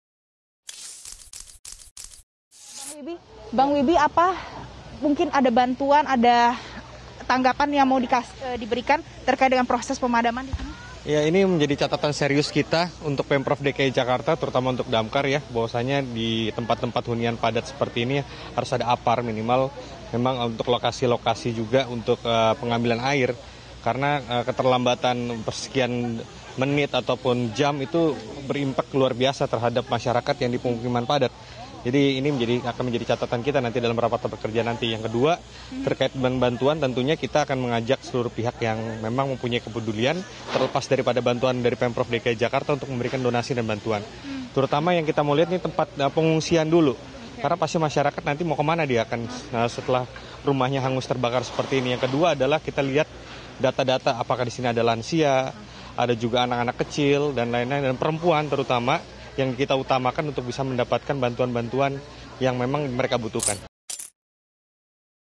Proses Pemadaman Api di Kawasan Simprug Sempat Terkendala Akses dan Air
Kebakaran permukiman padat penduduk terjadi di Simprug, Kebayoran Lama, Jakarta Selatan, pada Minggu (21/8/22). Petugas pemadam kebakaran sempat menemui sejumlah kendala untuk memadamkan api, salah satunya adalah lokasi kebakaran yang berada di jalan gang sempit dan jauh dari sumber air.

Politisi Partai NasDem, Wibi Andrino mengatakan proses pemadaman kebakaran menjadi catatan untuk Pemprov DKI Jakarta terutama untuk Damkar agar menyediakan APAR atau lokasi pengambilan air. Sebab, menurutnya, keterlambatan pemadaman akan memberikan dampak yang luar biasa.